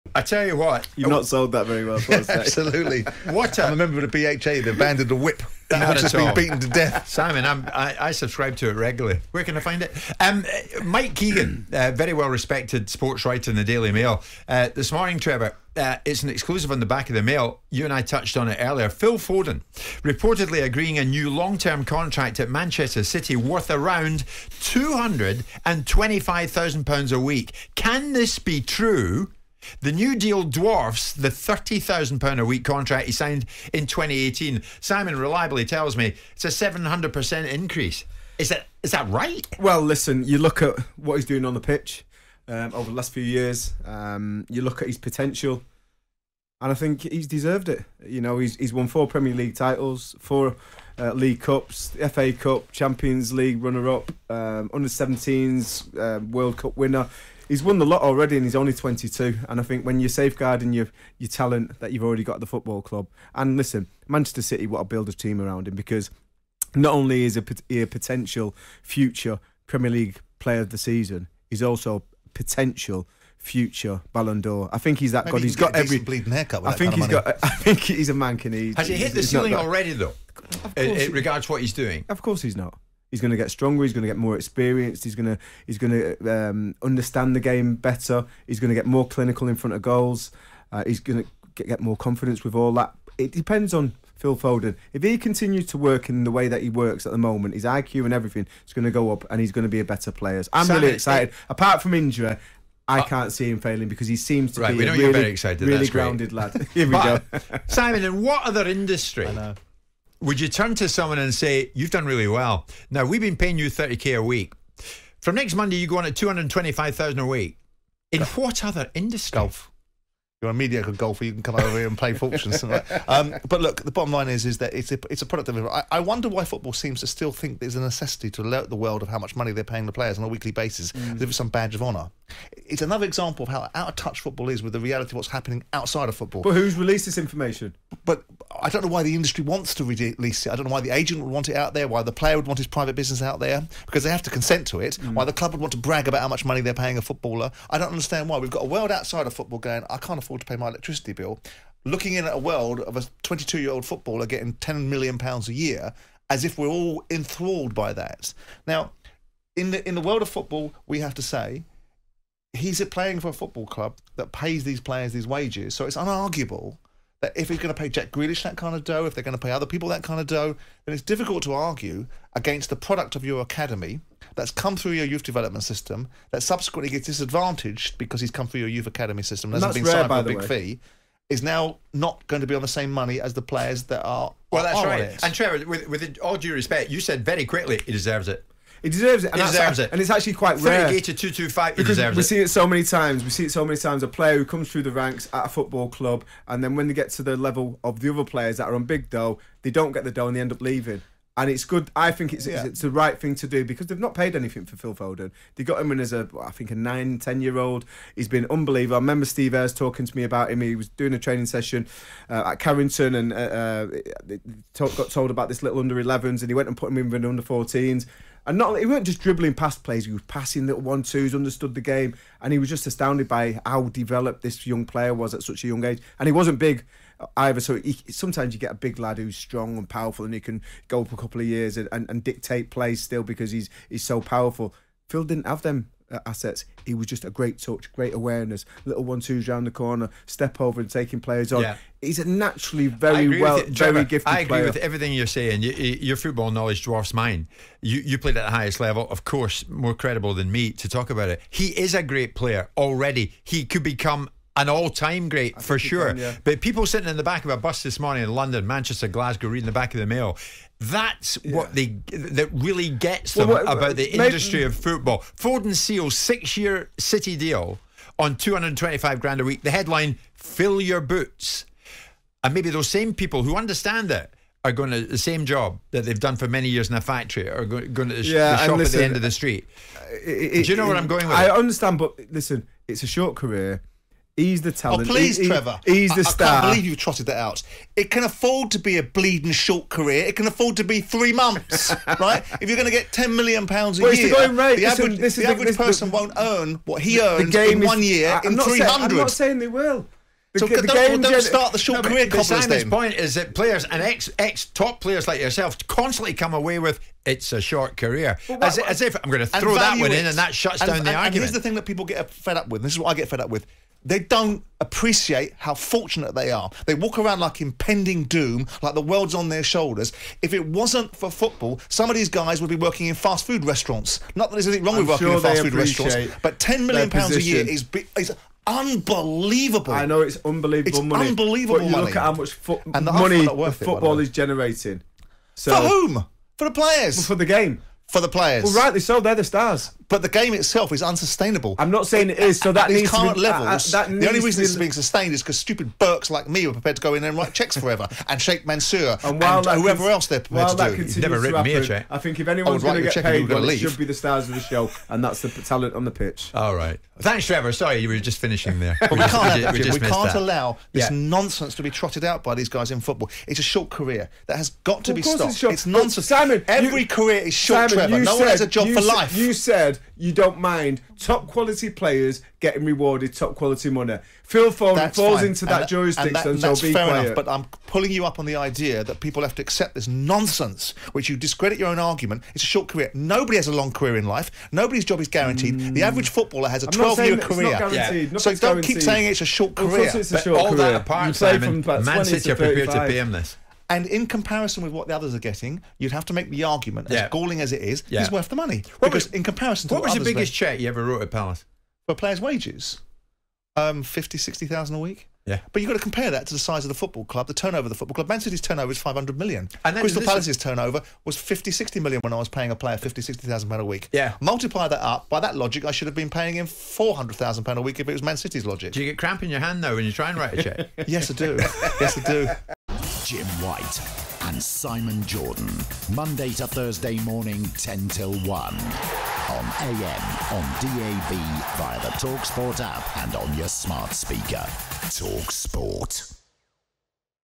Simon Jordan & Trevor Sinclair clash over Phil Foden's new Manchester City contract! 💰🔥
Phil Foden is reportedly set to sign a new deal worth £225k a week at Man City. Simon Jordan & Trevor Sinclair debate the figures!